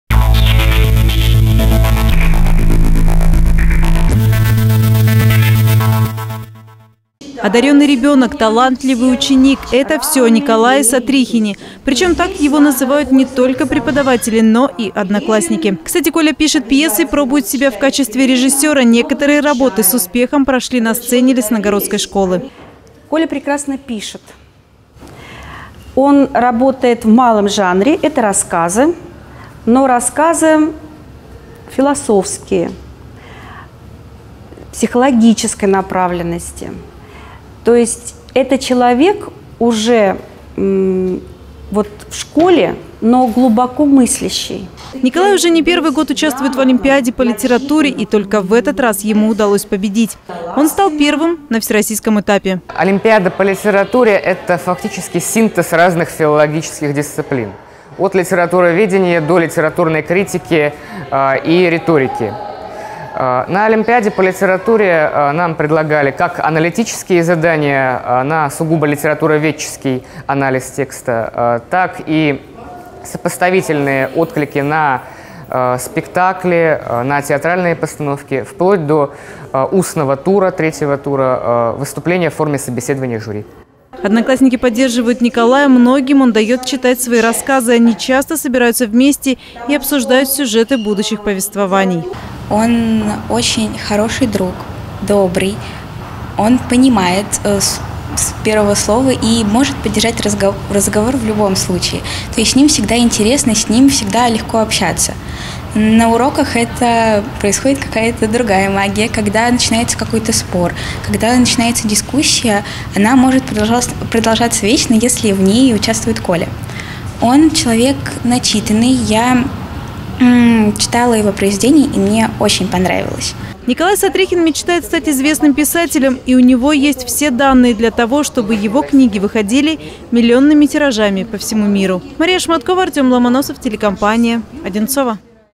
Первый в стране. Итоги олимпиады по литературе
Рассказы и новеллы: ученик Лесногородской школы Николай Сотрихин предпочитает малые жанры. Недавно молодой писатель стал победителем Всероссийской олимпиады среди школьников по литературе.